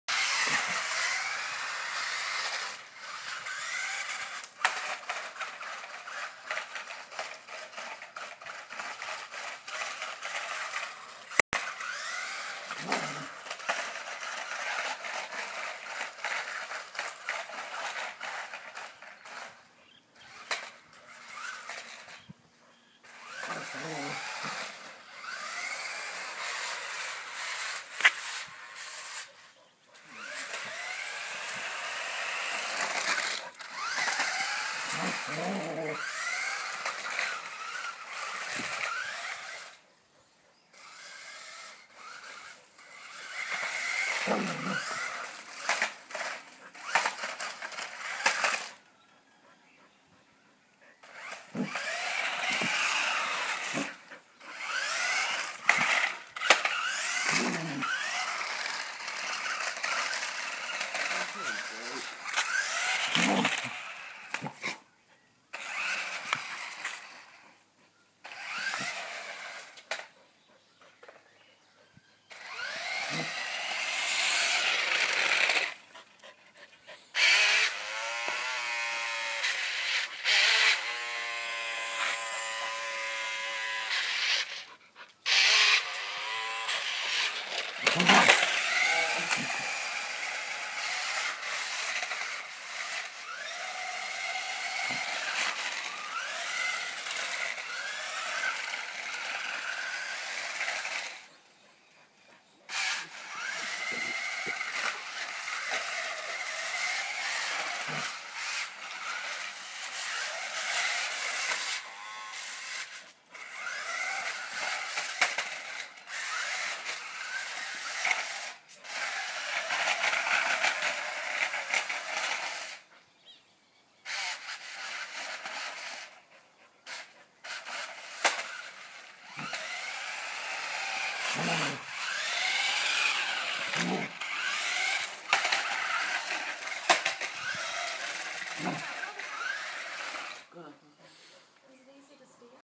Exercising the dog
The boys got a remote control car at the show. Shinook just can't help himself has got to round it up.. lol